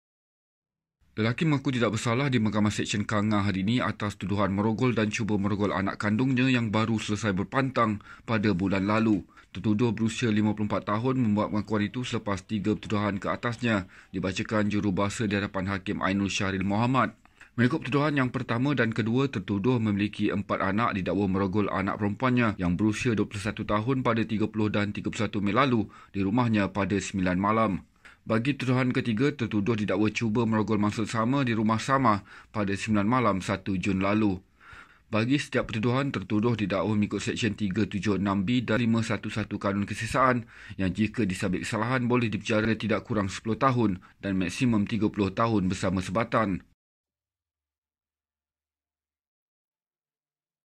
Lelaki didakwa rogol anak lepas pantang
KANGAR: Seorang lelaki mengaku tidak bersalah di Mahkamah Sesyen di sini, hari ini, atas tuduhan merogol dan cuba merogol anak kandungnya yang baru selesai berpantang, bulan lalu.